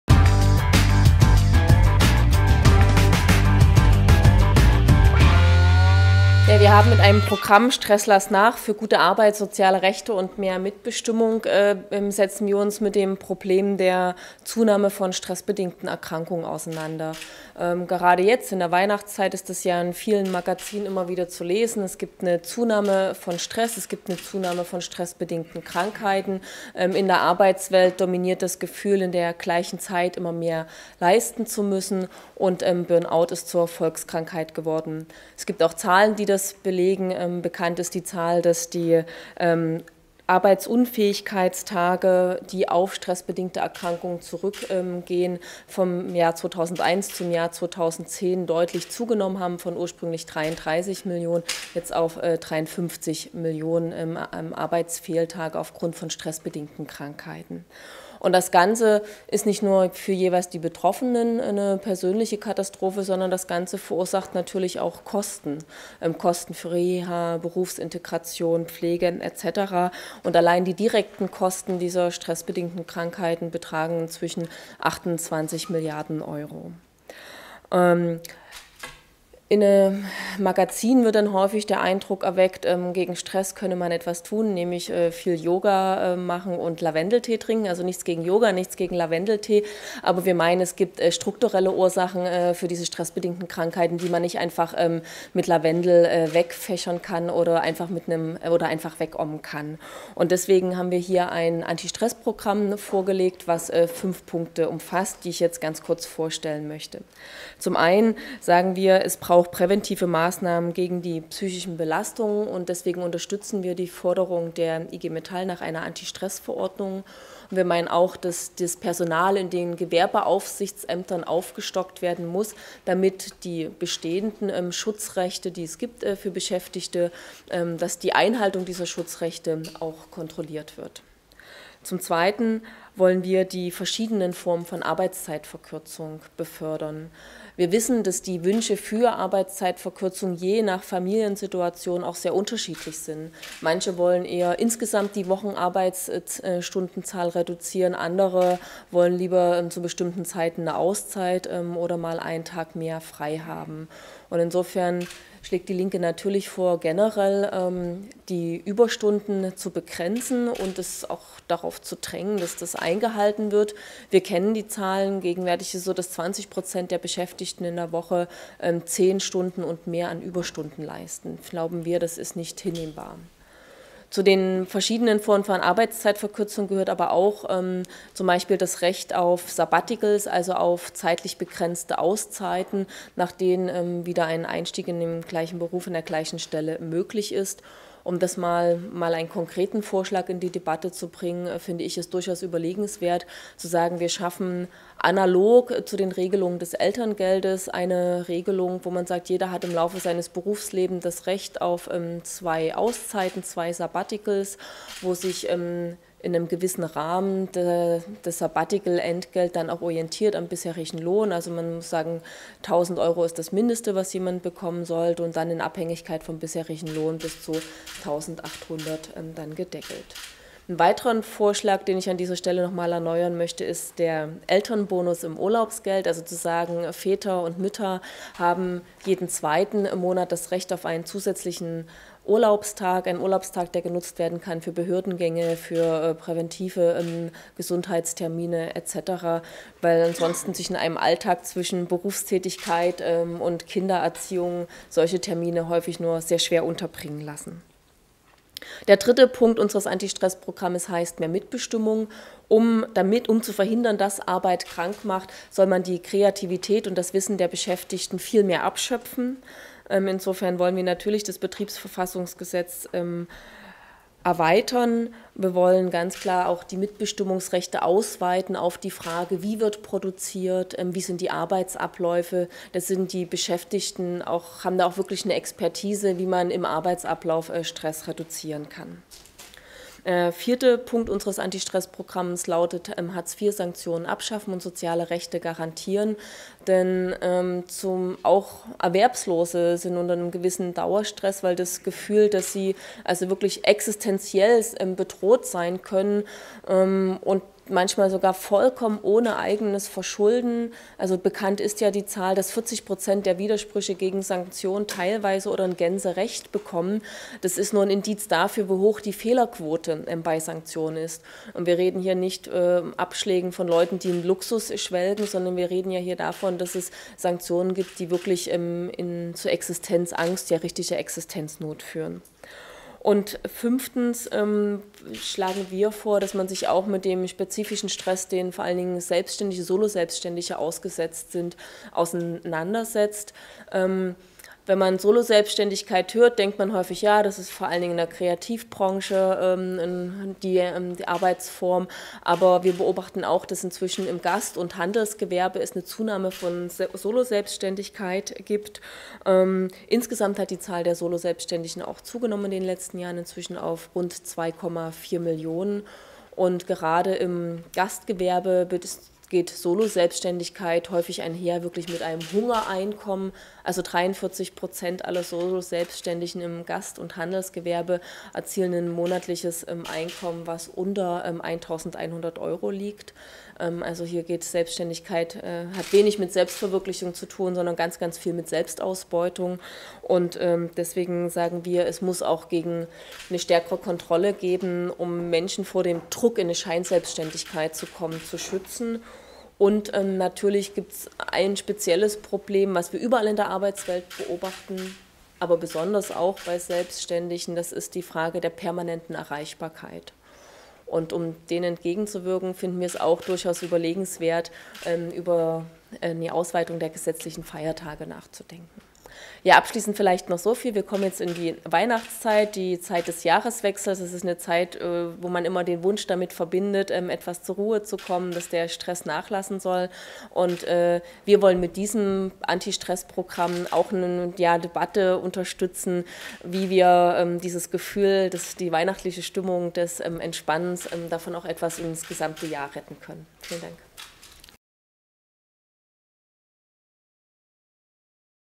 Stress lass nach! Für gute Arbeit, soziale Rechte und mehr Mitbestimmung
Psychische Belastungen nehmen in der Arbeitswelt durch steigenden Leistungsdruck und schlechte Arbeitsbedingungen drastisch zu. Immer mehr Arbeit muss in der gleichen Zeit erledigt werden. Arbeit am Abend oder am Wochenende, Überstunden und ständige Erreichbarkeit werden zur Normalität. Katja Kipping hat deshalb auf der Pressekonferenz im Berliner Karl-Liebknecht-Haus ein Fünf-Punkte-Programm zur Reduzierung der psychischen Belastungen in der Arbeitswelt vorgestellt.